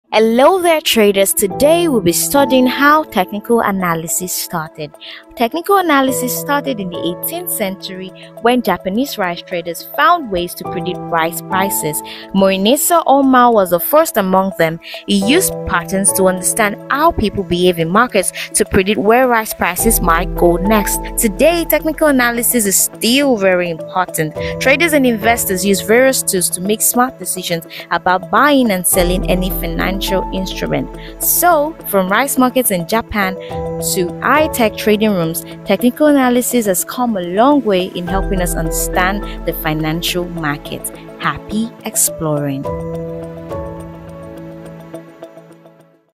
The Origins of Technical Analysis
Course: Technical Analysis for Everybody by FT9ja
Section 1: Technical Analysis 101
Video 3: The Origins of Technical Analysis

In the 18th century, Japanese rice traders, led by Munehisa Homma, pioneered technical analysis to predict rice prices.

Over time, traders enhanced their techniques using math and computers to analyze price movements.

Today, technical analysis remains crucial, aiding traders and investors in making informed decisions across financial markets.